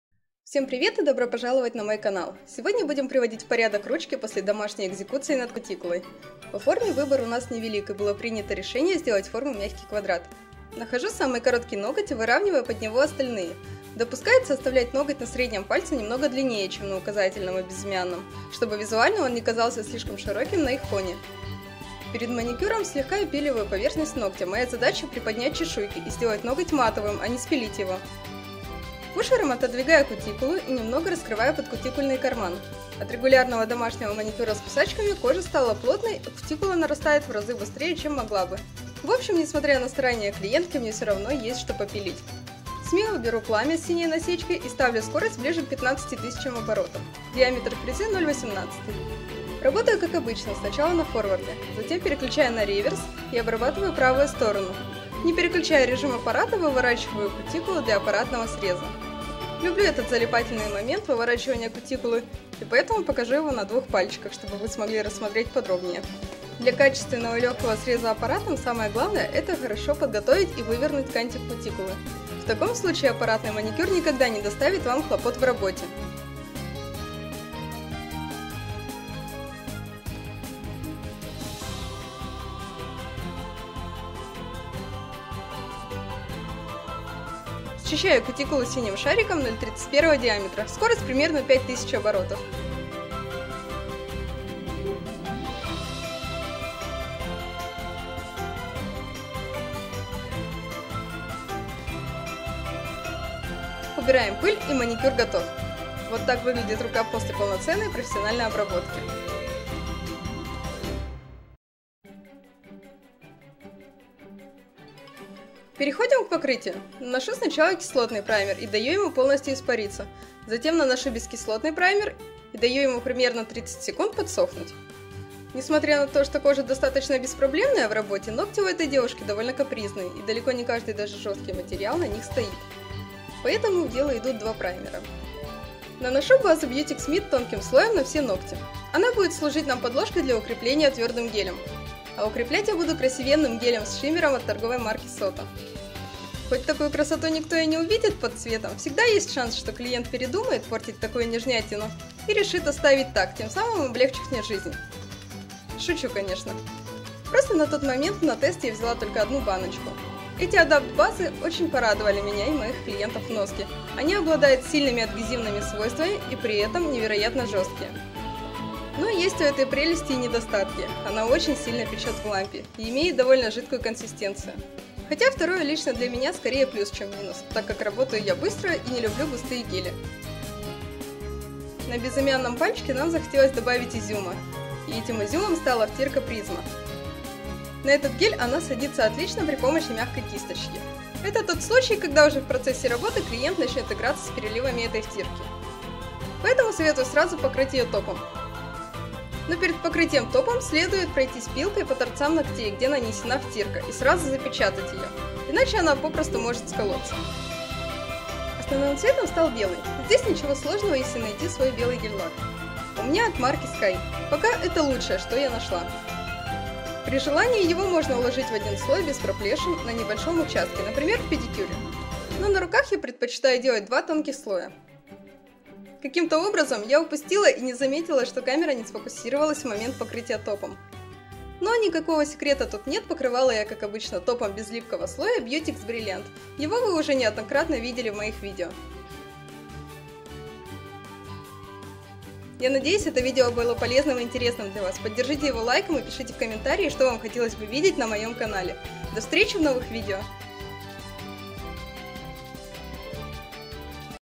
ИСПРАВЛЯЮ ДОМАШНИЙ МАНИКЮР. Аппаратный маникюр. Укрепление ногтей. Nail design. Hardware manicure
Мои соц. сети:
В этом видео будем исправлять ошибки домашнего классического маникюра при помощи аппаратной техники. Познакомимся с новыми гелями от торговой марки SOTA, немного расскажу об их плюсах и минусах, а так же моё личное впечатление от работы с ними.

In this video, we will correct the mistakes of a home classic manicure using hardware technology. Let's get acquainted with the new gels from the SOTA trademark, tell a little about their pros and cons, as well as my personal impression of working with them.
Материалы, которыми я работаю:
Кислотный и бескислотный праймер NeoNail
База Beautix MID
Топ Beautix Brilliant
Гель Adapt base Tm SOTA
Белый гель- лак Tm SKY
Металлическая основа и файл-пилка Zinger
Втирка призма
Все материалы, оборудование и реквизит были куплены на собственные средства. Рекламы в видео нет.

Спасибо за внимание, творческих успехов и хорошего дня!